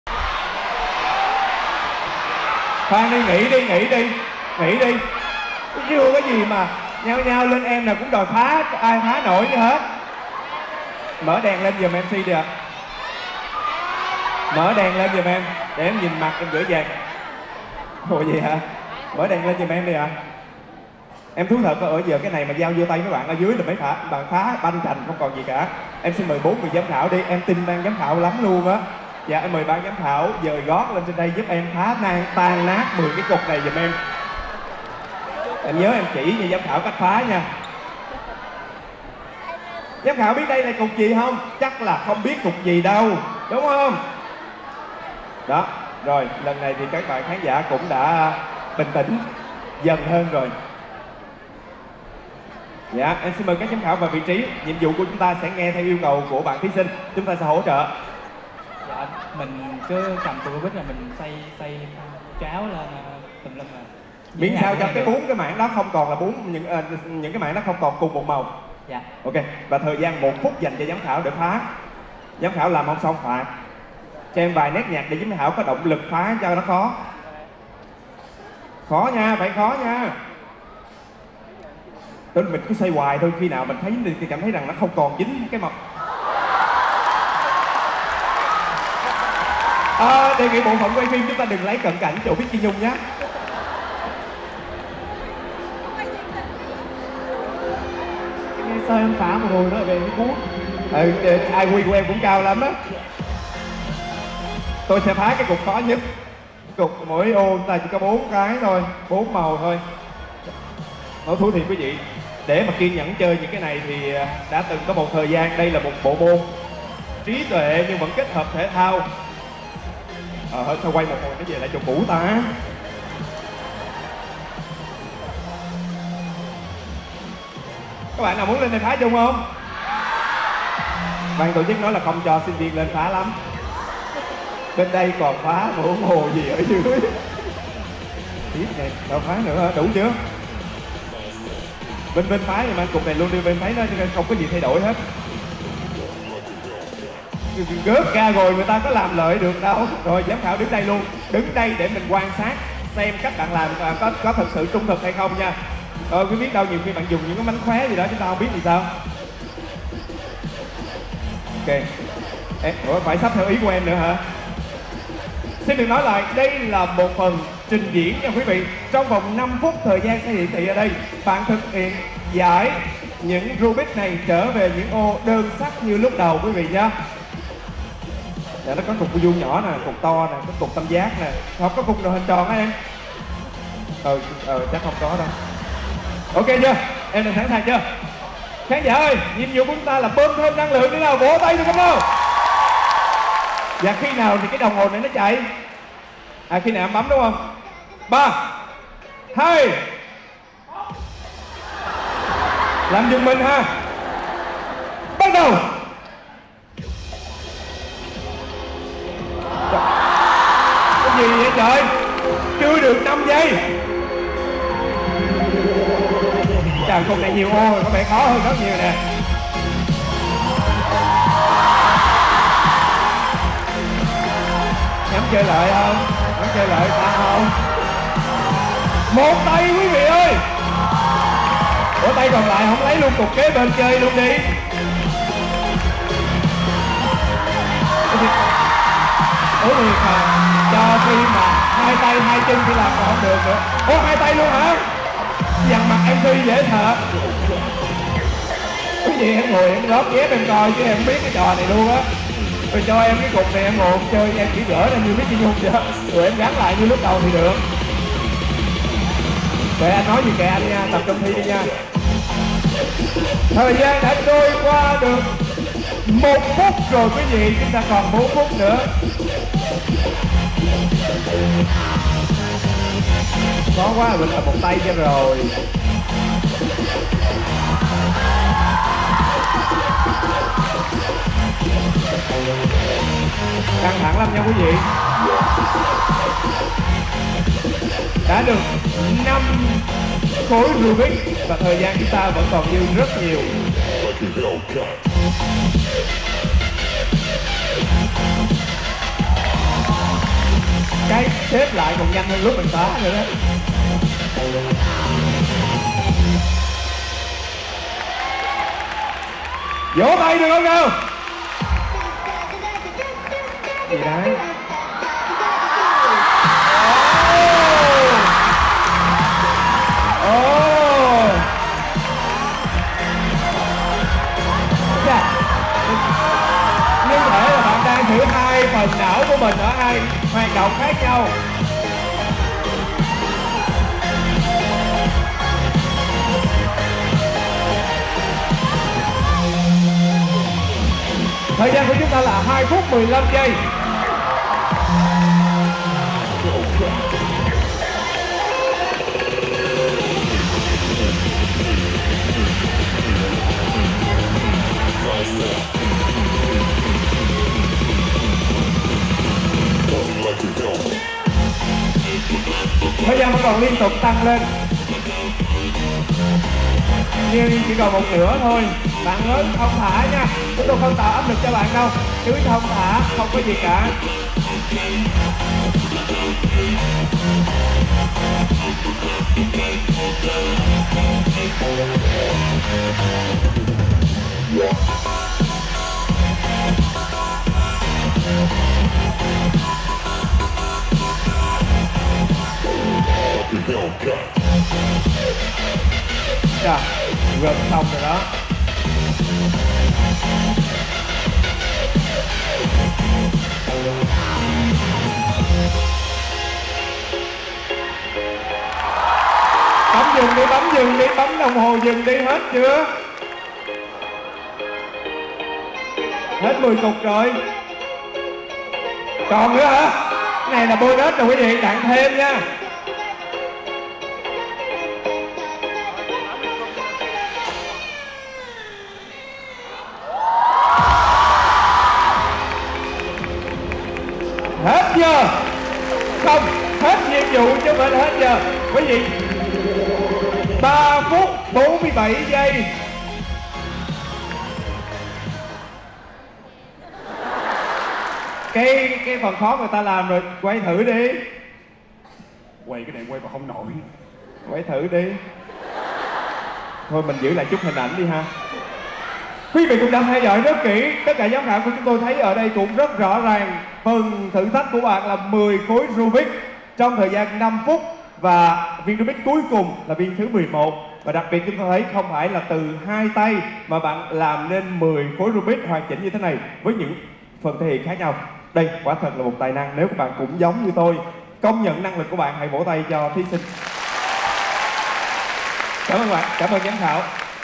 [VLU] LÊ HÀ PHONG | RUBIK | VANLANG'S GOT TALENT 2020
Chung kết cuộc thi VanLang's Got Talent 2020
🌟 Lê Hà Phong - SBD: 1077
🌟 Bài dự thi: RUBIK
✅ Giải ẤN TƯỢNG